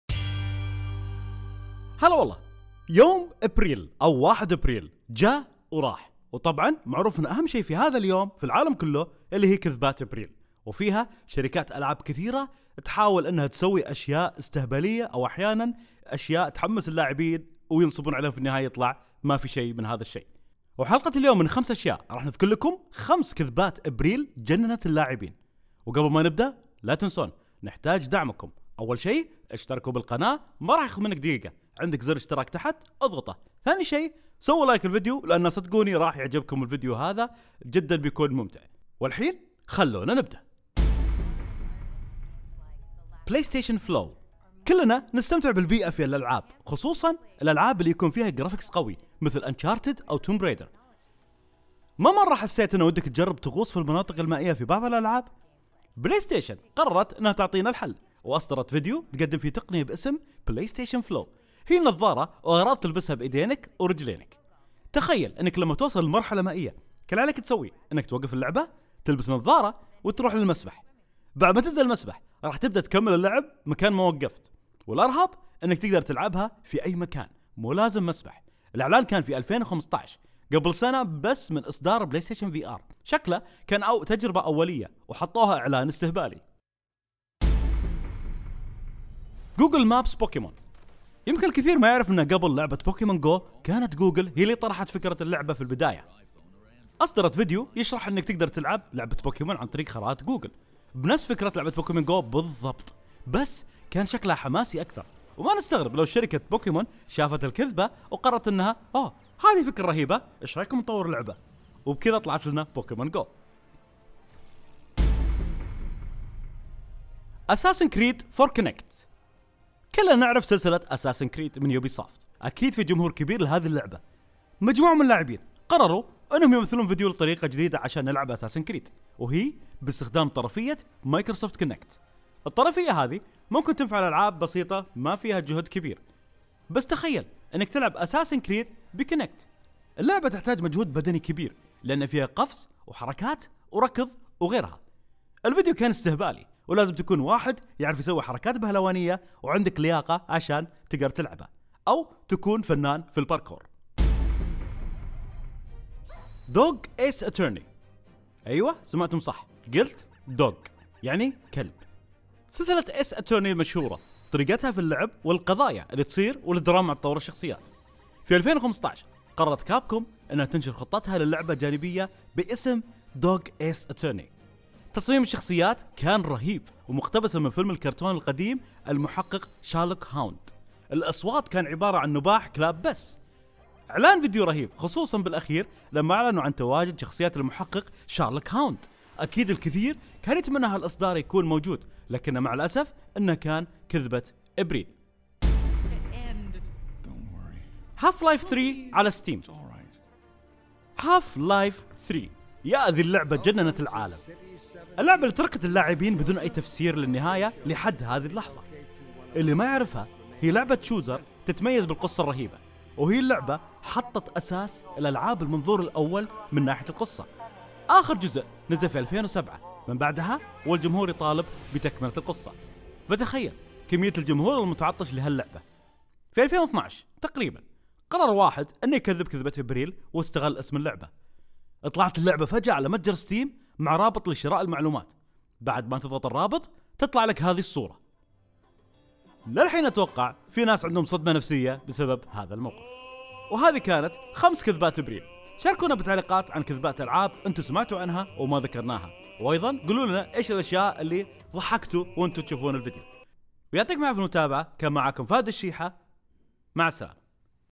5 كذبات ابريل جننت االلاعبيين
جبنا لكم ٥ كذبات ابريل جننت اللاعبين في الالعاب